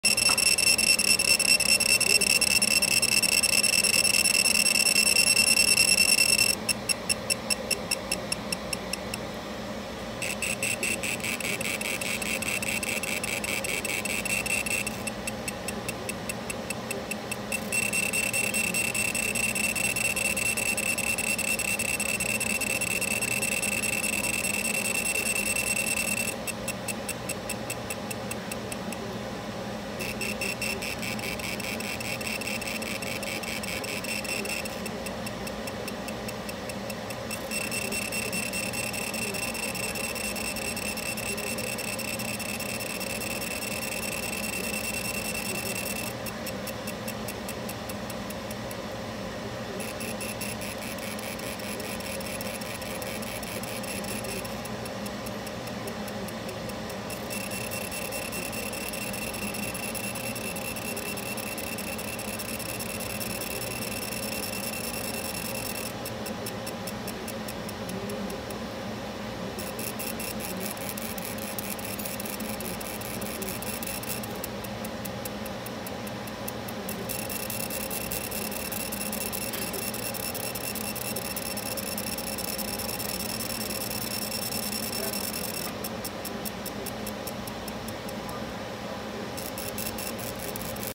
Laser etching a 1911 shroud
Bianchi Cup shroud being laser engraved